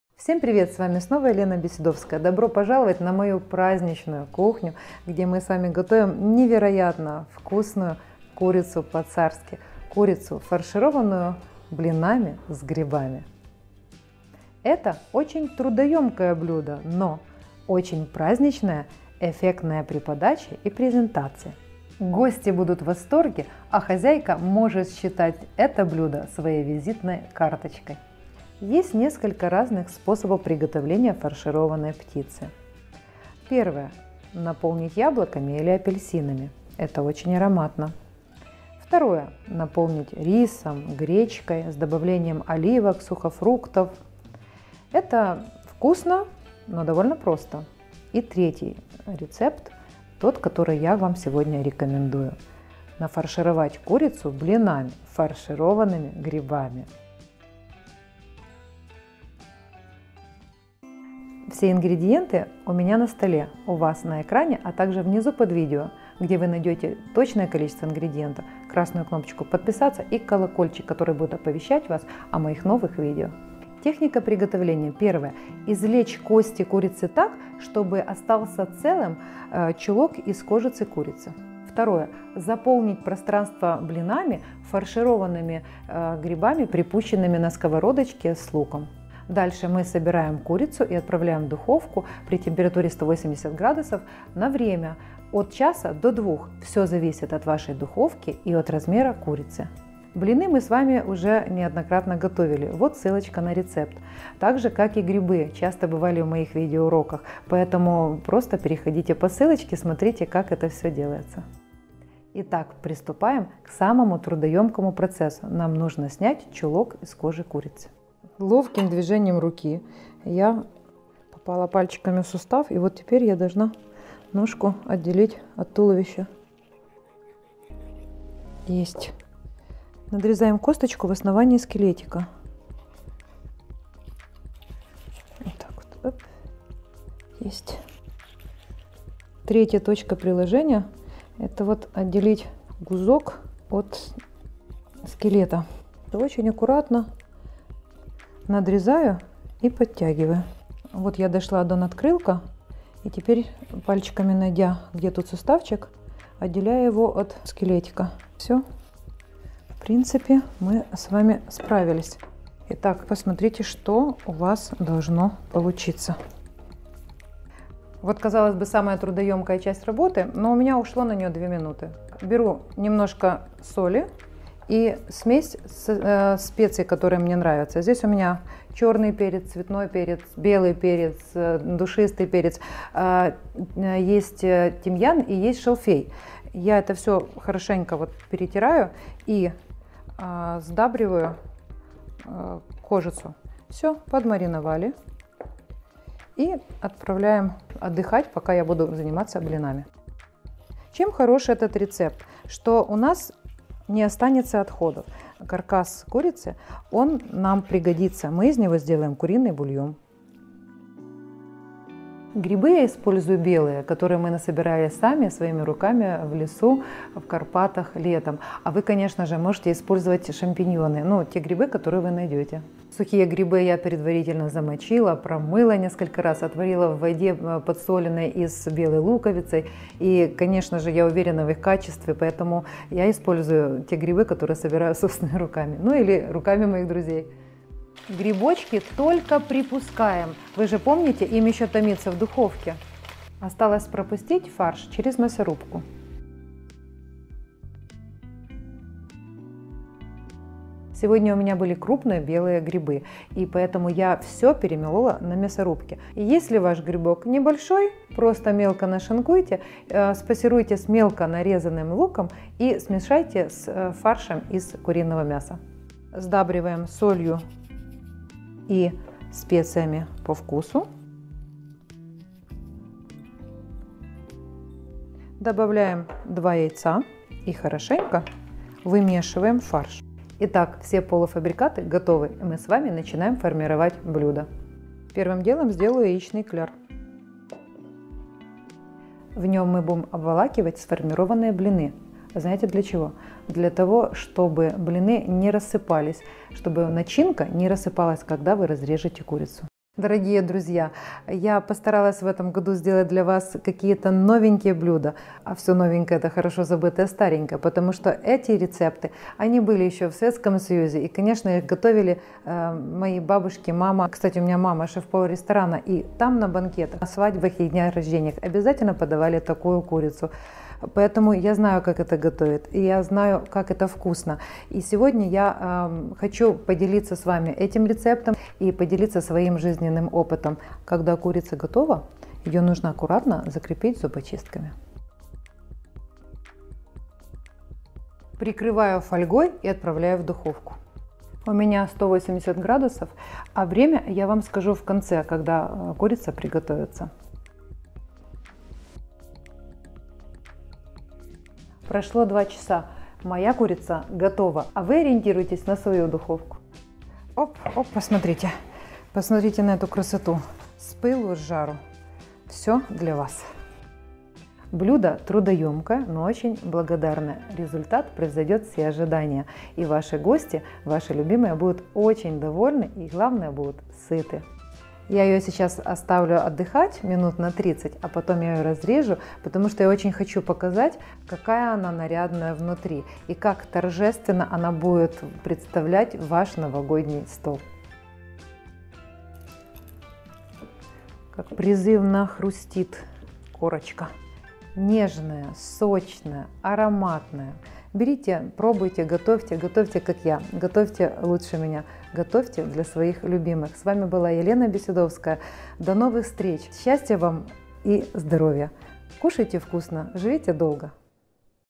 Курица фаршированная блинами с грибами
Рецепт фаршированной курицы:
- 1 курица
- 2 луковицы
- 5 яиц
- 100 мл. растительного масла
- 300 гр. белых грибов
- 9 блинов